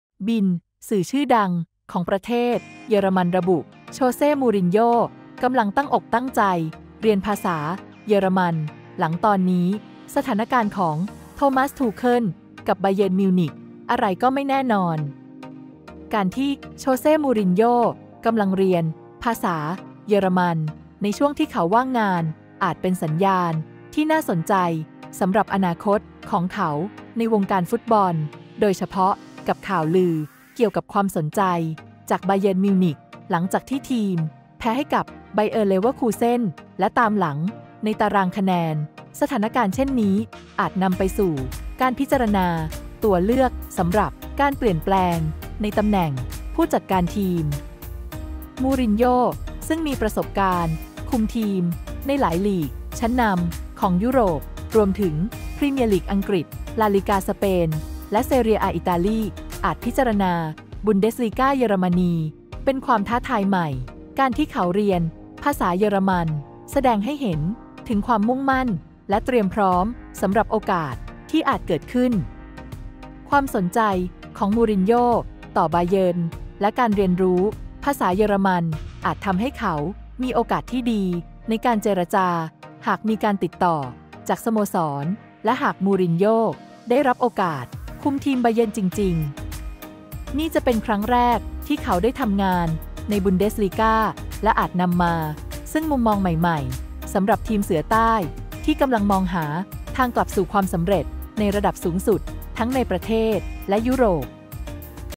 แค่ทักมาเท่านั้น!สื่อตีกระแส โชเซ่ มูริญโญ่ ศึกษาภาษาเยอรมัน อาจคุม บาเยิร์น
บิลด์ สื่อชื่อดังของประเทศเยอรมัน ระบุ โชเซ่ มูริญโญ่ กำลังตั้งอกตั้งใจเรียนภาษาเยอรมัน หลังตอนนี้สถาณการณ์ของ โทมัส ทูเคิ่ล กับ บาเยิร์นมิวนิค อะไรก็ไม่แน่นอน

ข่าวกีฬาวันนี้ รายงานว่า การที่โชเซ่ มูรินโญ่ กำลังเรียนภาษาเยอรมันในช่วงที่เขาว่างงานอาจเป็นสัญญาณที่น่าสนใจสำหรับอนาคตของเขาในวงการฟุตบอล โดยเฉพาะกับข่าวลือเกี่ยวกับความสนใจจากบาเยิร์น มิวนิค หลังจากที่ทีมแพ้ให้กับไบเออร์ เลเวอร์คูเซ่น และตามหลังในตารางคะแนน สถานการณ์เช่นนี้อาจนำไปสู่การพิจารณาตัวเลือกสำหรับการเปลี่ยนแปลงในตำแหน่งผู้จัดการทีม
มูรินโญ่ ซึ่งมีประสบการณ์คุมทีมในหลายลีกชั้นนำของยุโรป รวมถึงพรีเมียร์ลีก อังกฤษ, ลาลีกา สเปน, และ เซเรีย อา อิตาลี, อาจพิจารณาบุนเดสลีกา เยอรมนี เป็นความท้าทายใหม่ การที่เขาเรียนภาษาเยอรมันแสดงให้เห็นถึงความมุ่งมั่นและเตรียมพร้อมสำหรับโอกาสที่อาจเกิดขึ้น
ความสนใจของมูรินโญ่ ต่อบาเยิร์น และการเรียนรู้ภาษาเยอรมัน อาจทำให้เขามีโอกาสที่ดีในการเจรจาหากมีการติดต่อจากสโมสร และหากมูรินโญ่ ได้รับโอกาสคุมทีม บาเยิร์น จริงๆ นี่จะเป็นครั้งแรกที่เขาได้ทำงานในบุนเดสลีกา และอาจนำมาซึ่งมุมมองใหม่ๆ สำหรับทีมเสือใต้ ที่กำลังมองหาทางกลับสู่ความสำเร็จในระดับสูงสุดทั้งในประเทศและยุโรป